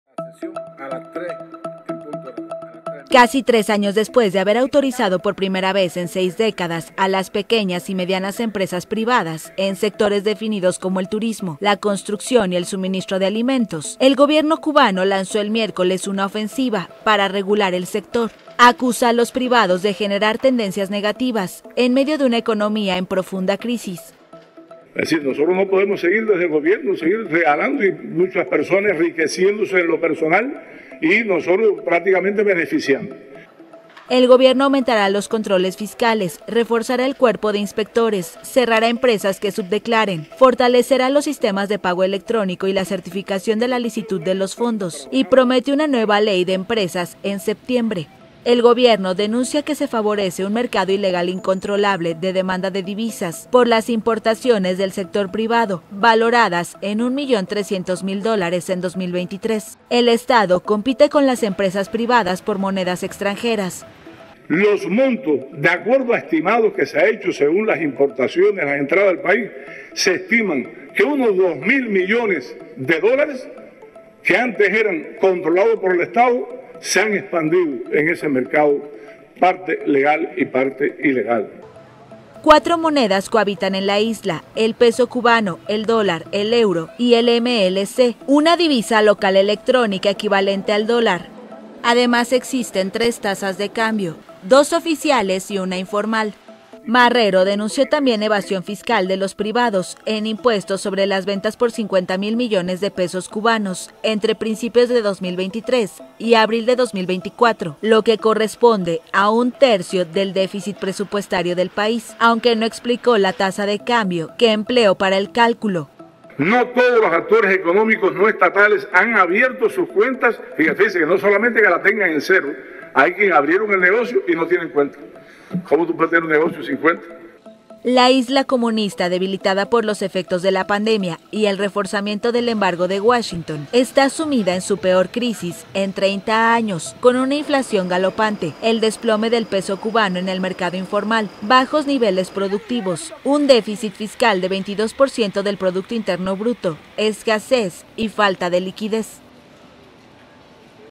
CUBA: gobierno anuncia DOLARIZACIÓN en TIENDAS y TURISMO para recuperar ingresos | Gestión
Mano dura contra la "corrupción" y la "evasión fiscal": casi tres años después de haber autorizado las pequeñas y medias empresas privadas, el gobierno cubano lanzó este miércoles una ofensiva para regular un sector que ha estremecido una economía en profunda crisis y al que acusa de generar "tendencias negativas".